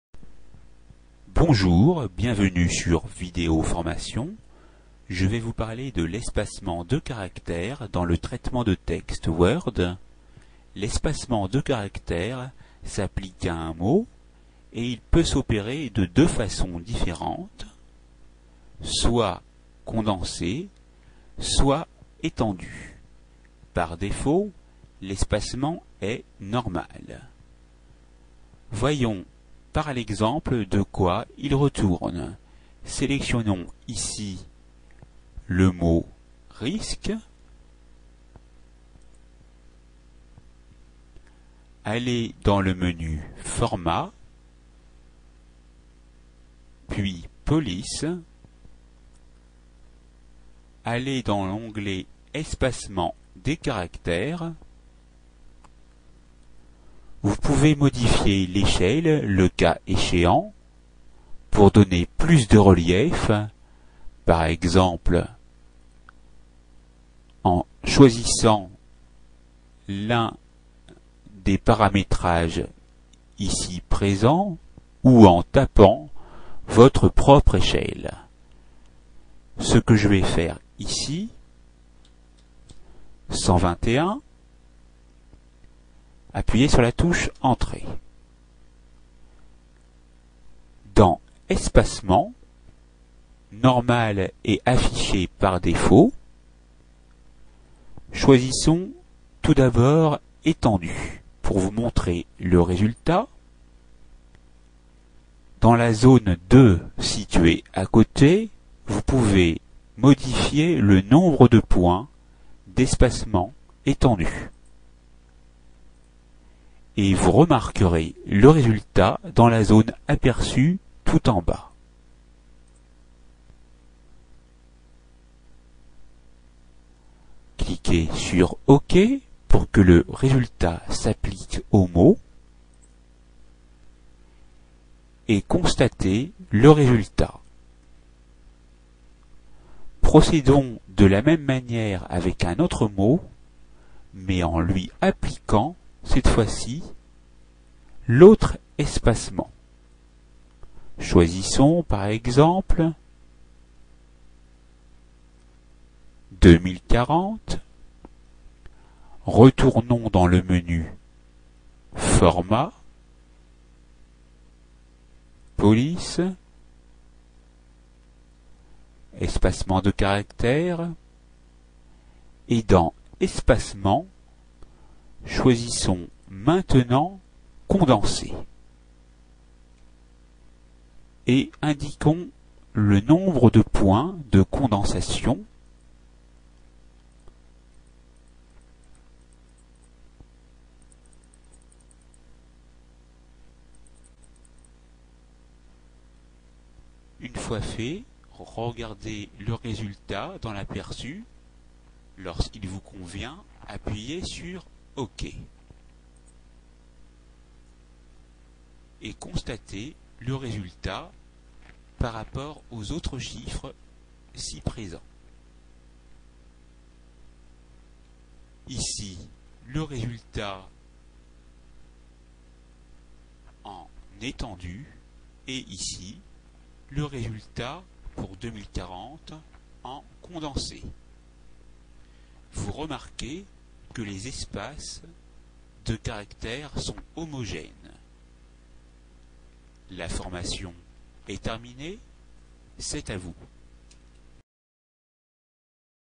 Les espaces de caractères dans Word
Dans ce tutoriel vidéo d'informatique et de bureautique sur le logiciel de traitement de texte Microsoft Office Word, je vais vous montrer comment modifier les espaces de caractères dans un mot dans votre document actif.